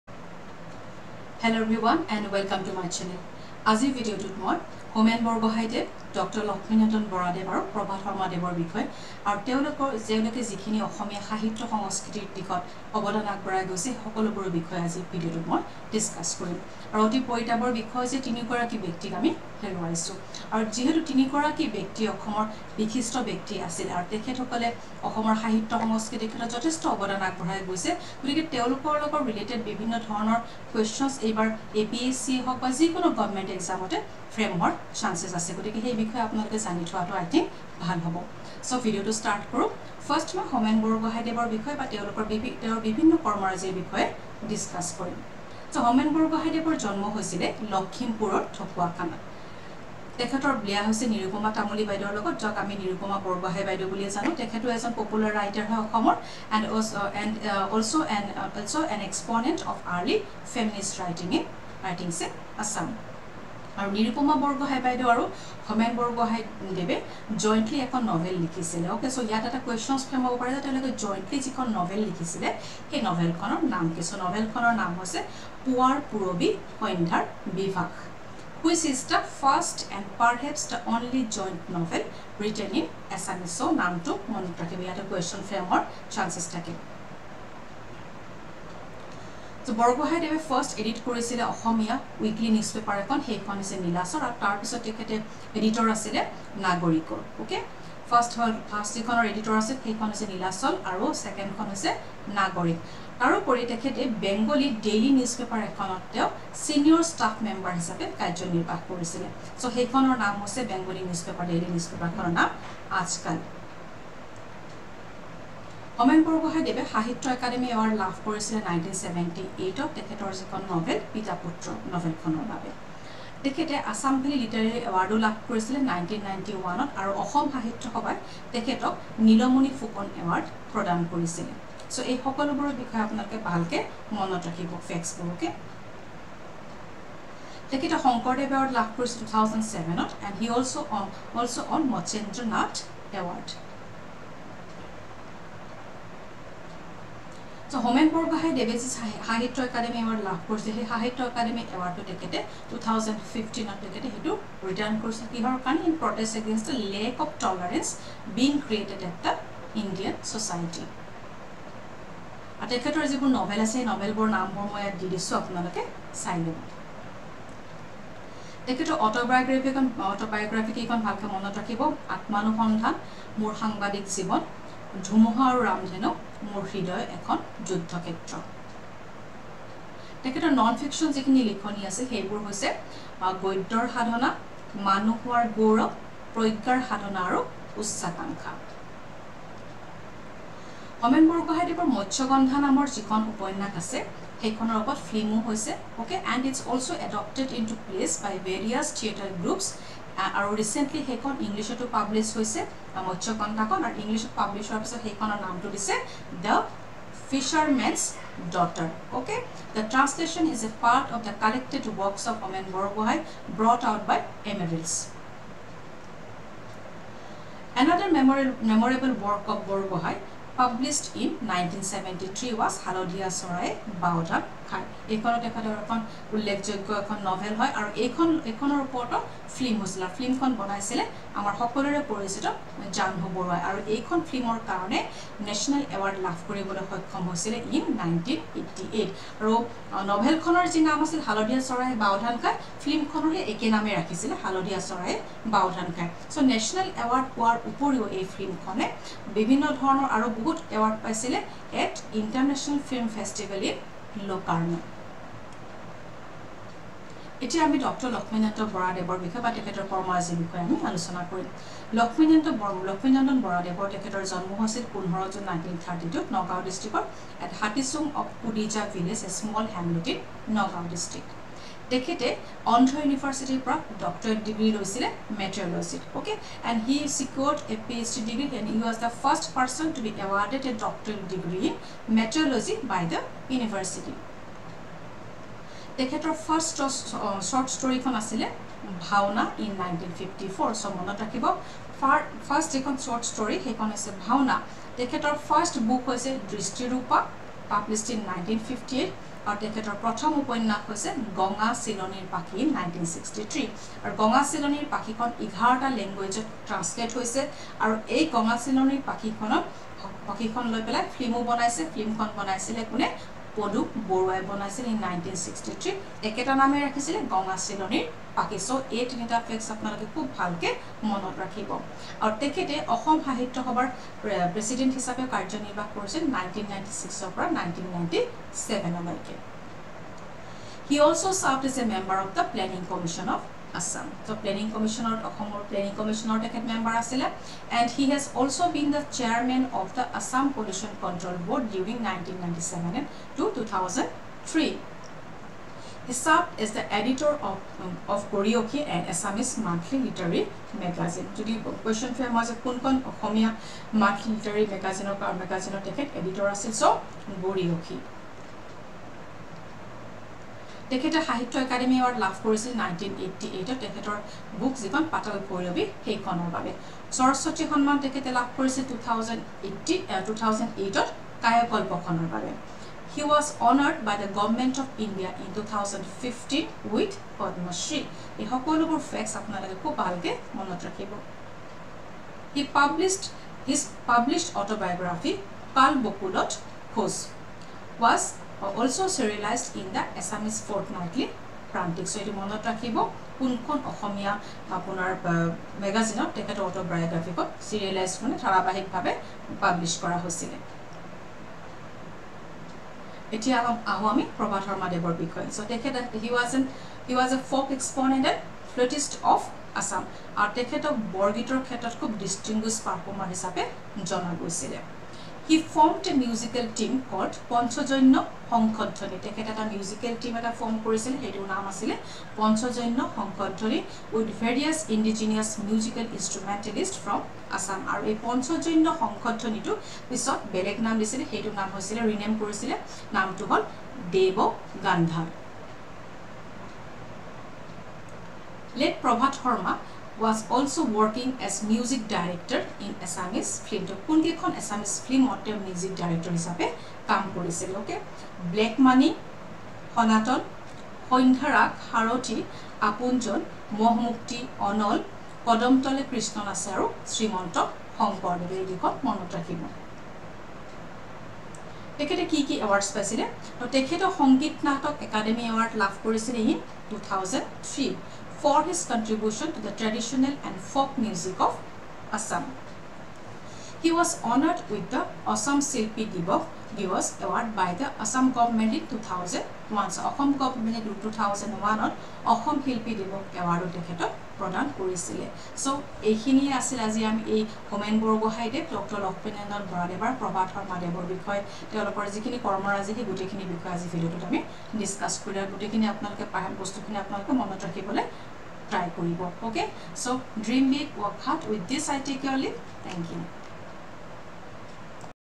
Homen Borgohai, Dr. Laxminandan Bora and Prabhat Sarma
Homen Borgohain, Dr. Laxminandan Bora and Prabhat Sarma.
Their contribution in the field of Assamese literature , art and culture.